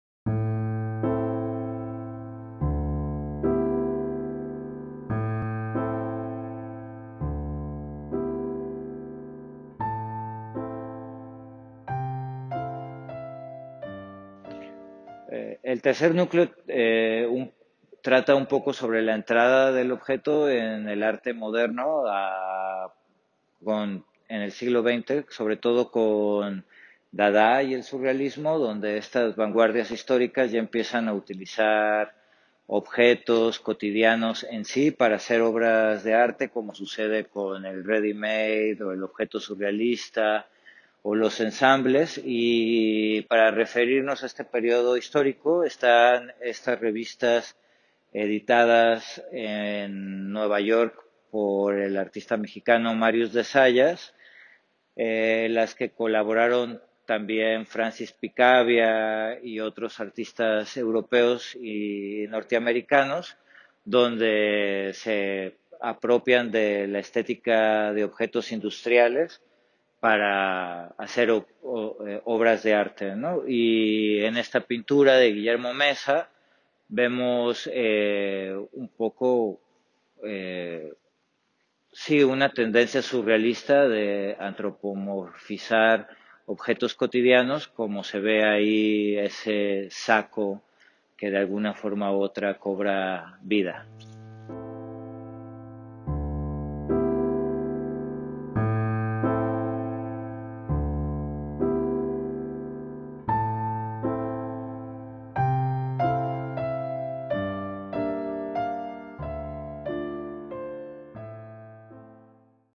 El objeto entra en escena
El dadá y el surrealismo fueron dos vanguardias históricas europeas que realmente tuvieron un impacto y mantuvieron un diálogo internacional. En relación al objeto y su presencia en la obra de arte, son primordiales. Con el dadá y el surrealismo, el objeto entra en escena y su uso se sistematiza con fines críticos. Tal es el caso de Marcel Duchamp quien expuso por primera vez un readymade en la ciudad de Nueva York: Fountain , 1917 (en la actualidad existe un debate sobre la autoría de esta pieza que involucra a Duchamp y Elsa von FreytagLoringhoven). El readymade inauguró el desconcierto sobre el objeto cotidiano en el arte al presentar un urinario firmado como una obra para ser presentada en una exposición.

Un colaborador cercano al grupo neoyorkino de dadá, en el que se encontraba Duchamp, fue el mexicano Marius de Zayas quien compartió parte del interés en los objetos de producción serial. El ocaso del dadá dio pie al surgimiento del surrealismo en 1924. Existieron algunos artistas, principalmente pintores, que en momentos se acercaron a la estética del surrealismo y acudieron a objetos con el fin de animarlos o darles un aspecto humano en sus imágenes, como sucede con la pintura de Guillermo Meza, Capa roja (s/f).